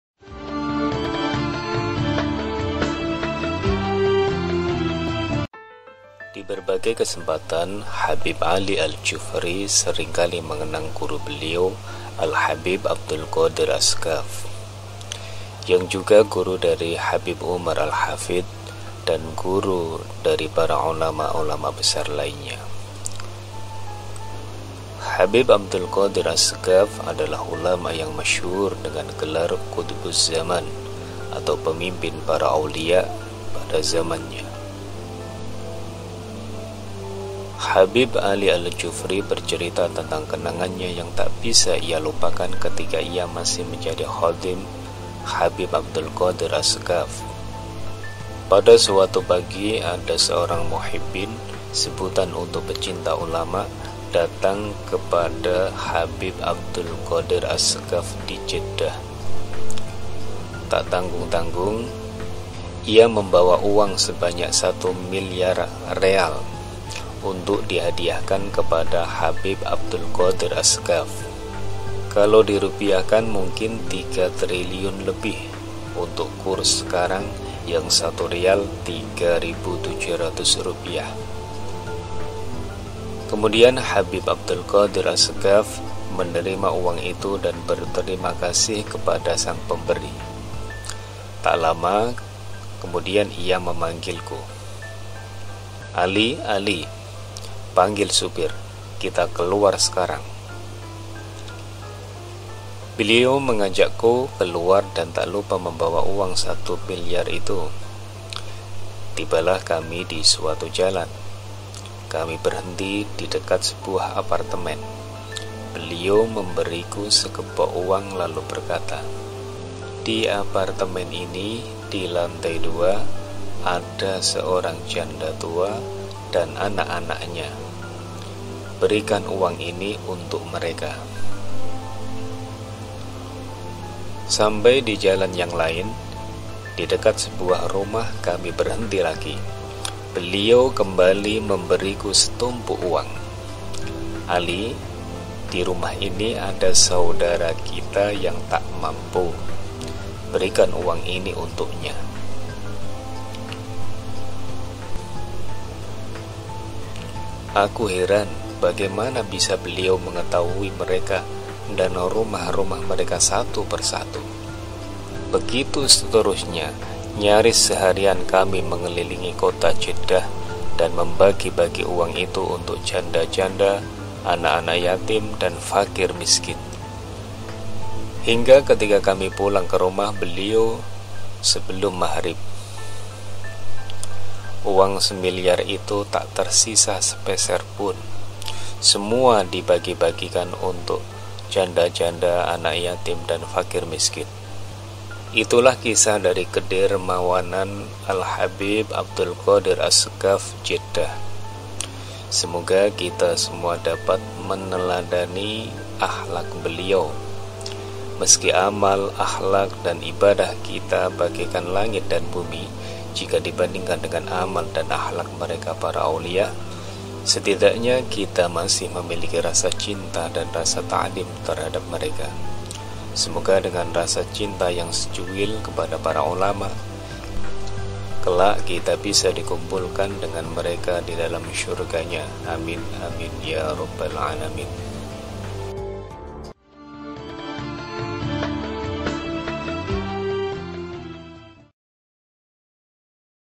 Wali Qutub Ini Sedekah Uang 1 Milyar Riyal
Habib Abdul Qadir Assegaf lahir pada Jumadil Akhir Tahun 1331 H dan pernah bermukim di Jeddah Saudi Arabia.
Salah satu kelebihan Habib Abdul Qadir ketika berdakwah , beliau banyak menerangkan hal-hal yang sebelumnya tidak banyak diketahui orang. Beliau juga sering melakukan hijrah dan perjalanan dakwah mulai dari Hadhramaut, Makkah, Jeddah, Singapura hingga datang ke Indonesia pada tahun 1974.
Habib Abdul Qadir pernah mengatakan, lemahnya keimanan disebabkan karena kebanyakan orang sekarang merasa berat bangun malam, lebih suka bermalas-malasan, ini semua jelas akibat bujuk rayu setan.

Di antara murid-murid beliau adalah Al-Habib Muhammad bin Abdullah Alhaddar, Al-Habib Zein bin Ibrahim Bin Sumaith, Al-Habib Salim bin Abdullah bin Umar Asy-Syathiry, Al-Habib Abubakar Al-'Adany bin Ali Al-Masyhur, Sayyid Muhammad bin Alwi Al-Maliki, ِAl-Habib Abubakar bin Hasan Al-Atthas dan masih banyak lagi.